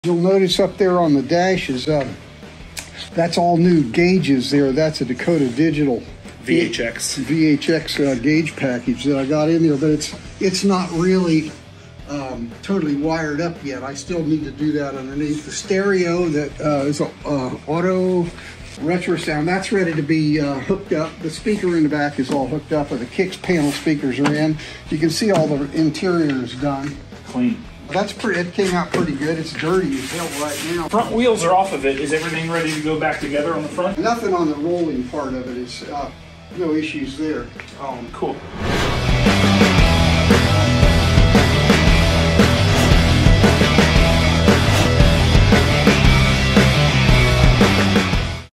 New Gauges for the 1962 Impala SS Convertible
Pop installed a new set of Dakota Digital VHX gauges in his '62 Impala SS barn find, and they look incredible! This is the perfect addition to any restomod.